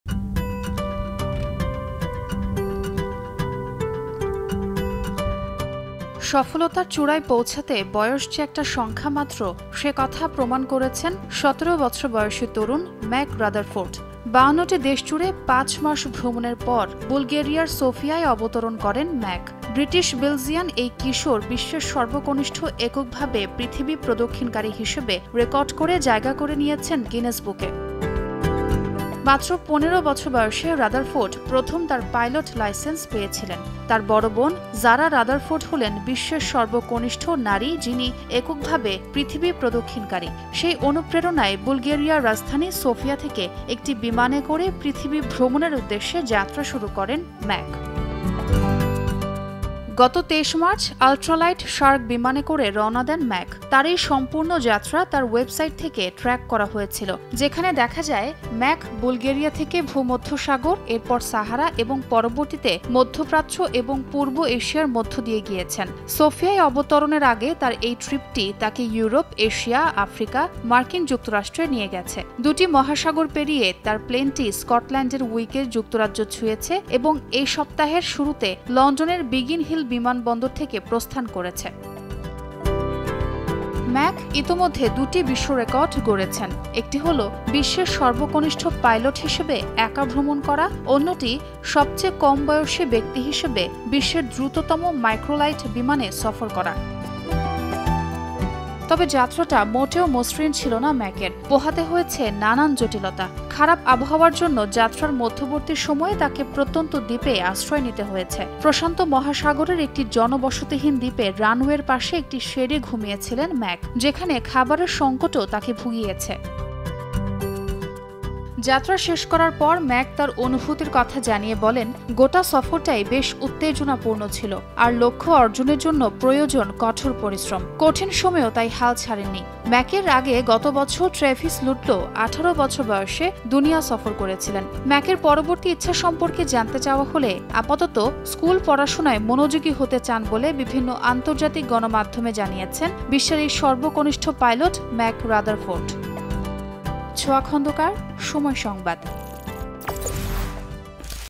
১৭ বছর বয়সে একা বিশ্ব ভ্রমণ! | Mack Rutherford | World Tour | Somoy TV
সফলতার চূড়ায় পৌঁছাতে বয়স যে একটা সংখ্যা মাত্র সে কথা প্রমাণ করেছেন ১৭ বছর বয়সী তরুণ ম্যাক রাদারফোর্ড। ৫২টি দেশ জুড়ে পাঁচ মাস ভ্রমণের পর বুলগেরিয়ার সোফিয়ায় অবতরণ করেন ম্যাক। ব্রিটিশ-বেলজিয়ান এই কিশোর বিশ্বের সর্বকনিষ্ঠ এককভাবে পৃথিবী প্রদক্ষিণকারী হিসেবে রেকর্ড করে জায়গা করে নিয়েছেন গিনেস বুকে।